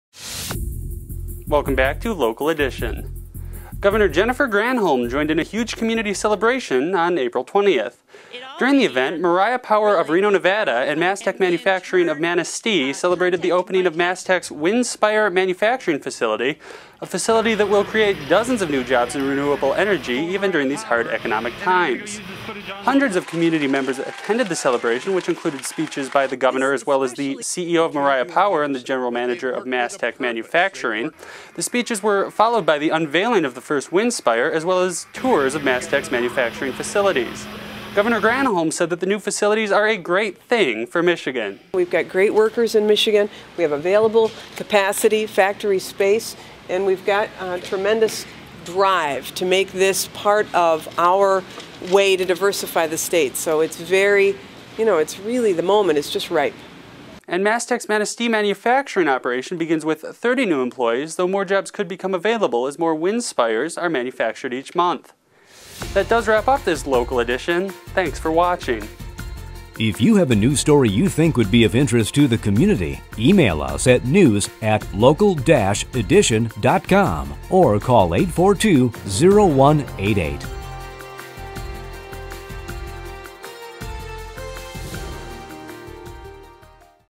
Governor Joins Community for Manufacturing Kick-Off
Michigan Governor Jennifer Granholm joined in a huge community celebration on April 20.

During the event, Mariah Power of Reno, Nevada, and MasTech Manufacturing of Manistee, celebrated the opening of MasTech’s Windspire Manufacturing Facility…. a facility that will create dozens of new jobs in renewable energy, even during these hard economic times.

Hundreds of community members attended the celebration, which included speeches by the Governor as well as the CEO of Mariah Power and the General Manager of MasTech Manufacturing. The speeches were followed by the unveiling of the first Windspire, as well as tours of MasTech’s manufacturing facilities.

“As we continue our efforts to diversify and grow our state’s economy, we are proud to celebrate the opening of the Windspire manufacturing facility and the new energy jobs this will create in northern Michigan,” said Governor Granholm. “This investment puts us at the forefront in the production of renewable energy to end U.S. dependence on foreign oil and fossil fuels.”
MasTech’s Manistee manufacturing operation begins with 30 new employees, though more jobs could become available as more Windspires are manufactured monthly.